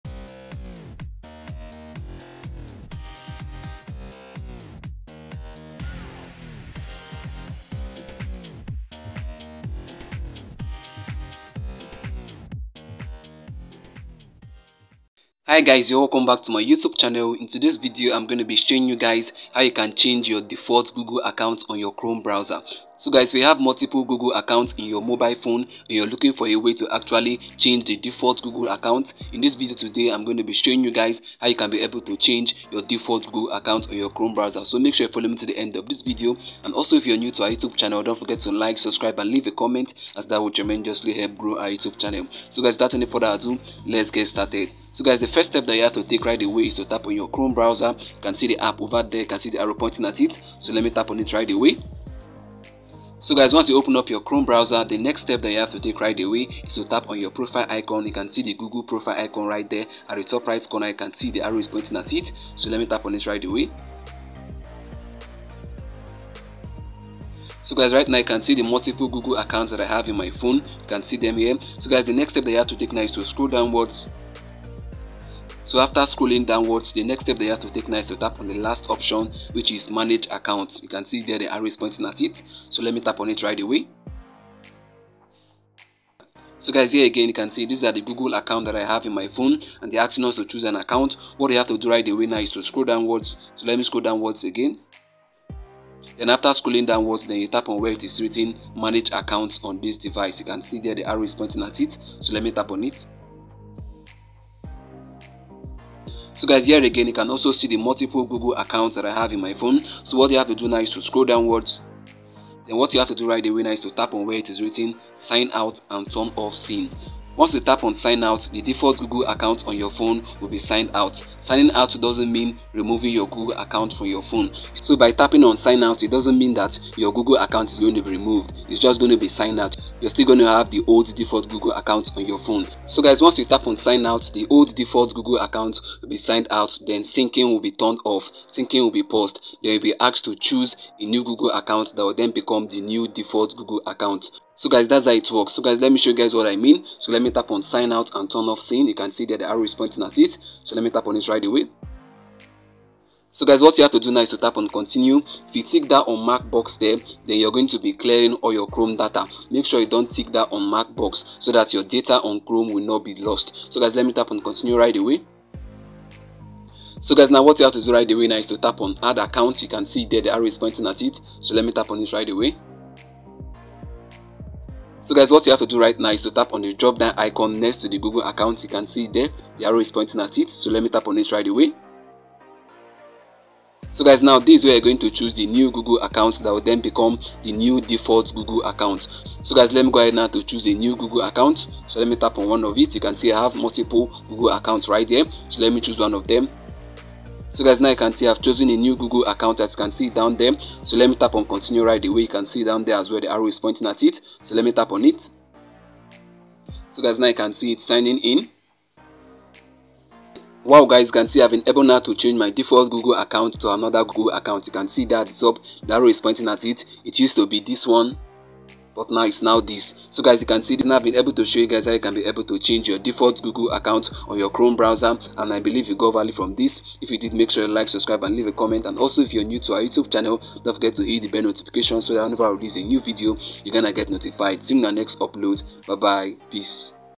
How To Change Default Google Account on Chrome Browser | Change Default Gmail Account on Browser
How do I change my default google account on Google Chrome browser on mobile? If that is the question you are looking for an answer to, then you are watching the right video, just make sure you watch to the end of this video to get it all. Below are the fews steps involved in changing your default google account on your browser:

✓Open your Google chrome browser.
✓Tap on the profile icon on the top- right corner of the screen.
✓Tap on manage account.
✓Tap on manage account on this device.
✓Tap on Sign out and off sync."
✓Now you can sign in with the account you want to set as the default account. Choose from a list of Google accounts or add one by clicking "Use another account."